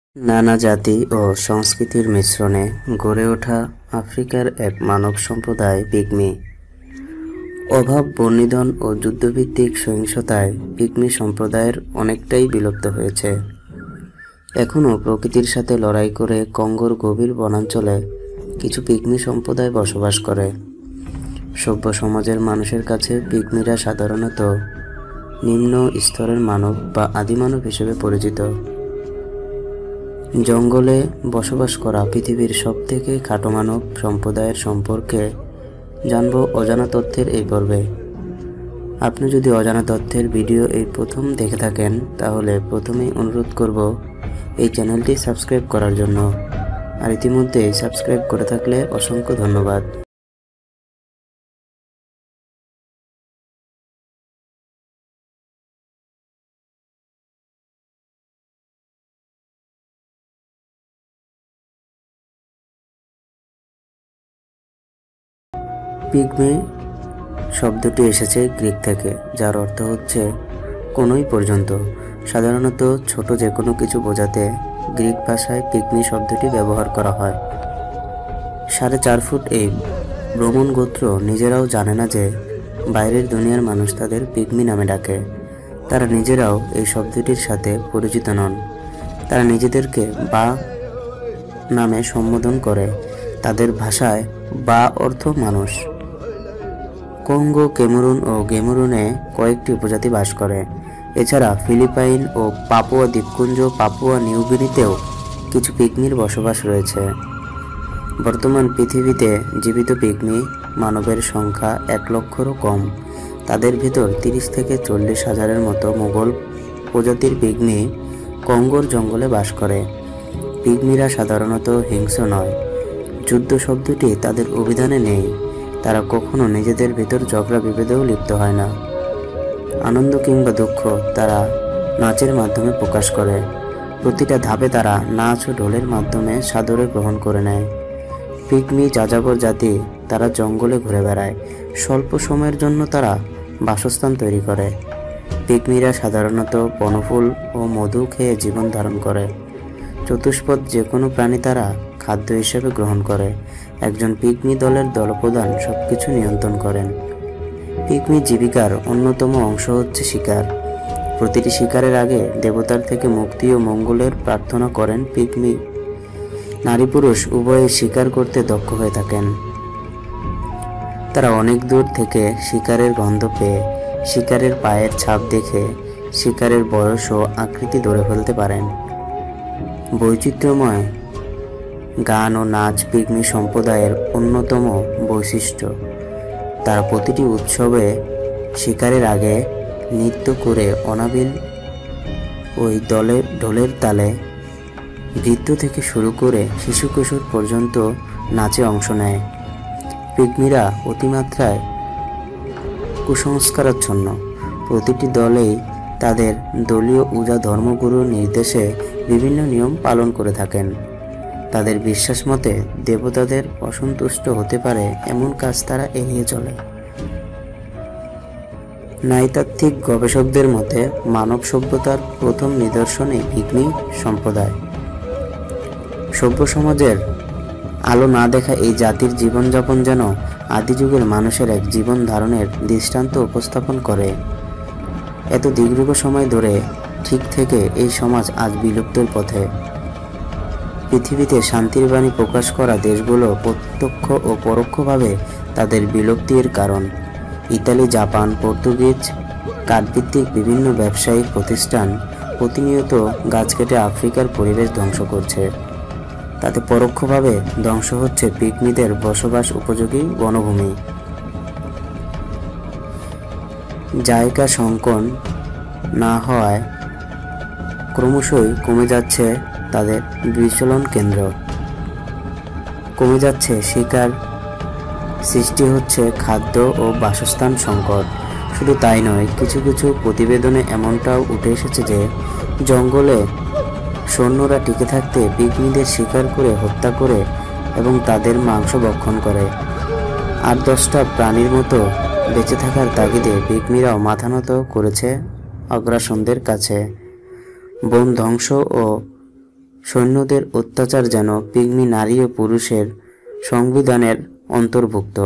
আধুনিক শহর থেকে বিতাড়িত পিগমি জাতি সম্পর্কে দেখুন | Pygmy| অজানা তথ্য
নানা জাতি, বর্ণ ও সংস্কৃতির মিশ্রণে গড়ে ওঠা আফ্রিকা মহাদেশের এক বিশেষ মানব সম্প্রদায় পিগমি জাতিগোষ্ঠী। শিকারের অভাব, বন নিধন এবং যুদ্ধভিত্তিক সহিংসতায় পিগমি সভ্যতার অনেকটাই বিলুপ্ত হয়েছে। তবে এখনো প্রতিকূল পরিবেশের সাথে লড়াই করে কঙ্গোর গভীর বনাঞ্চলে কিছু পিগমি সম্প্রদায় বসবাস করে। সভ্যসমাজের কাছে পিগমিরা সাধারণত নিম্নস্তর এর মানব বা বনমানুষ হিসেবে পরিচিত।